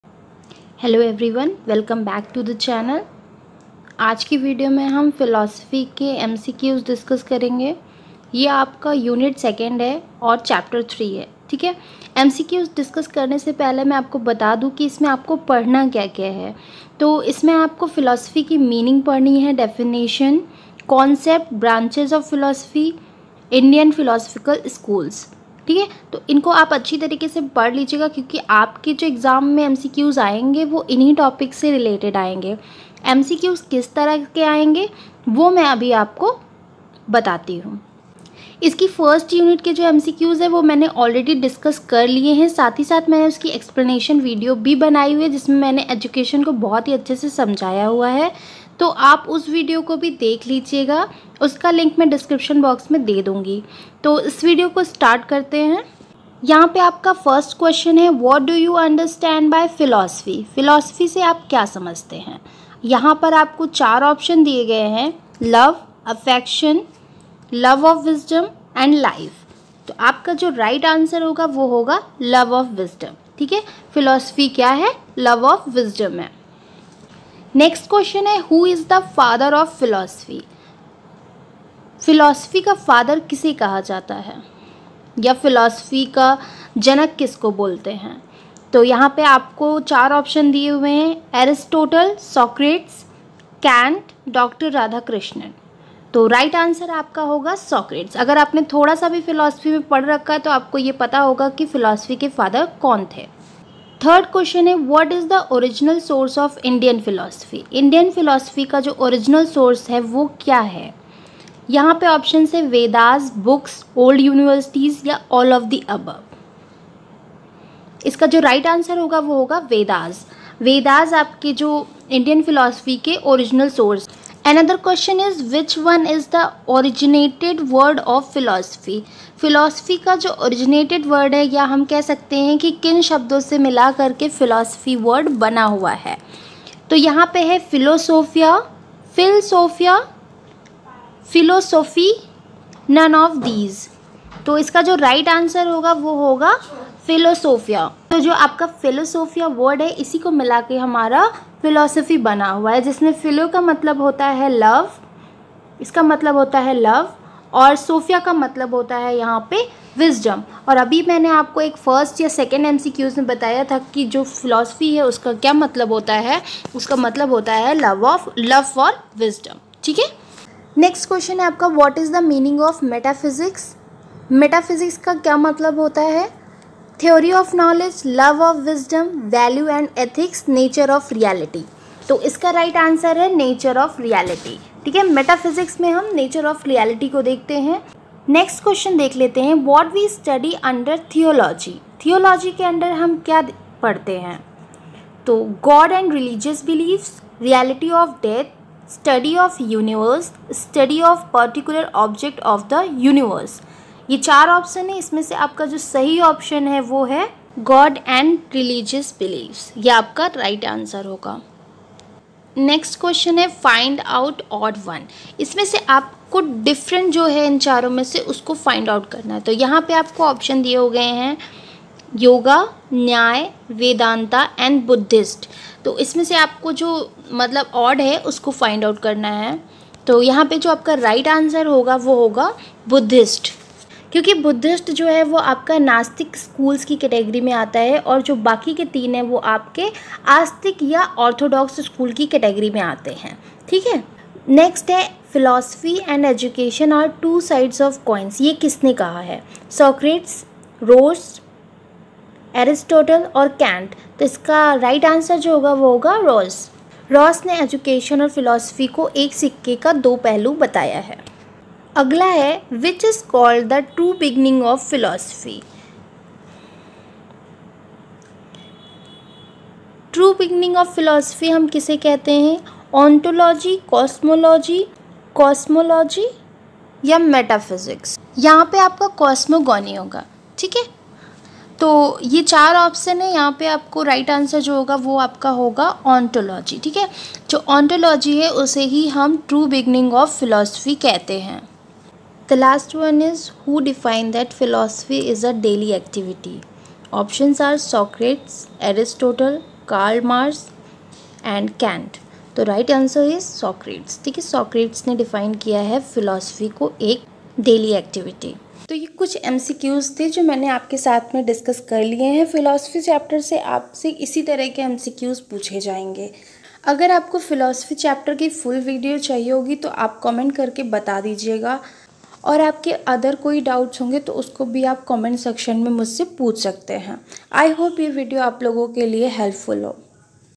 philosophy important mcqs || educationalphilosophy || let's revise
I have discussed important mcqs of philosophy
unit: 2
chapter: 3
chapter name : philosophy